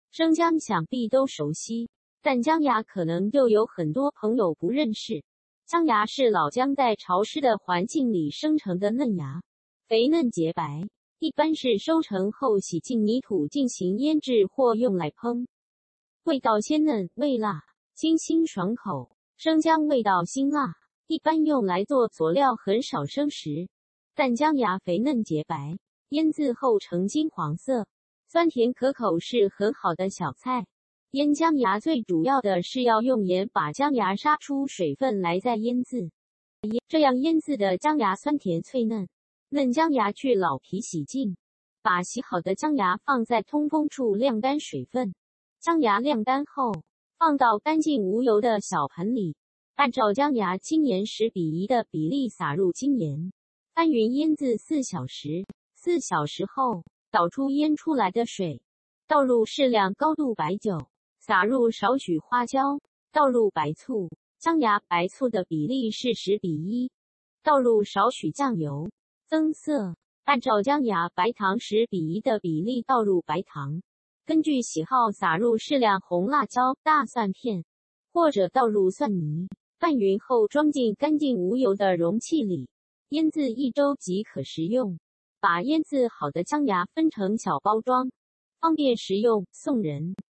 脆腌姜芽
生姜想必都熟悉，但姜芽可能就有很多朋友不认识。姜芽是老姜在潮湿的环境里生成的嫩芽，肥嫩洁白。 一般是收成后洗净泥土进行腌制或用来烹，味道鲜嫩、味辣，清新爽口。
生姜味道辛辣，一般用来做佐料很少生食。但姜芽肥嫩洁白，腌渍后呈金黄色，酸甜可口是很好的小菜。腌姜芽最主要的是要用盐把姜芽杀出水分来再腌渍，这样腌渍的姜芽酸甜脆嫩。
嫩姜芽去老皮洗净。
把洗好的姜芽放在通风处晾干水份。
姜芽晾干后，放到干净无油的小盆里，按照姜芽、精盐10:1的比例洒入精盐，翻匀腌渍4小时。
4小时后，倒出腌出来的水。
倒入适量高度白酒。
洒入少许花椒。
倒入白醋，姜芽、白醋的比例是10:1.
倒入少许酱油，增色。
按照姜芽、白糖10:1的比例倒入白糖
根据喜好洒入适量红辣椒、大蒜片，或者倒入蒜泥。
拌匀后装进干净无油的容器里，腌渍一周即可食用。
把腌渍好的姜芽分成小包装，方便食用、送人。